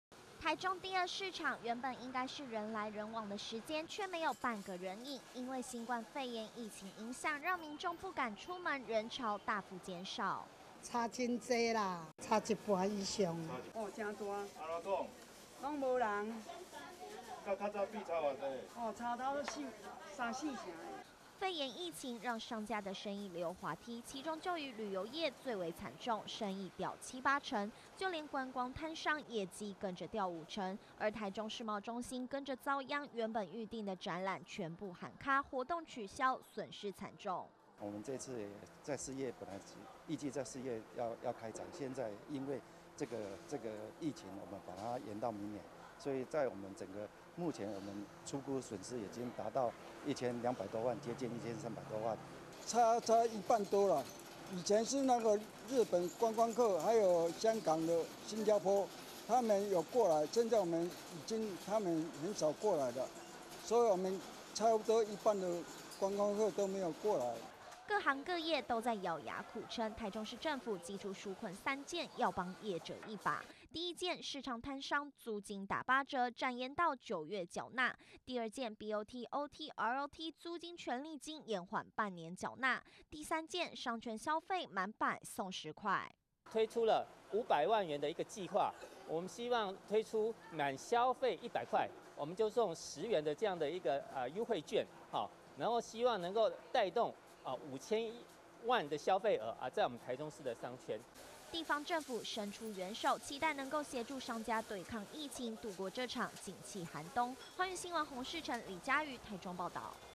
肺炎疫情重創觀旅業 台中”紓困三箭”相助｜寰宇新聞 20200220
【寰宇新聞】

疫情衝擊，消費力大減，現在為了幫助產業重生，也希望刺激人潮回流，台中市政府祭出”紓困三箭”，不只幫市場攤商租金打折，還推出消費滿百元折十元的措施，希望協助攤商度過難關，只是這波衝擊，恐怕還會持續蔓延到零售業、商圈、以及會展等產業。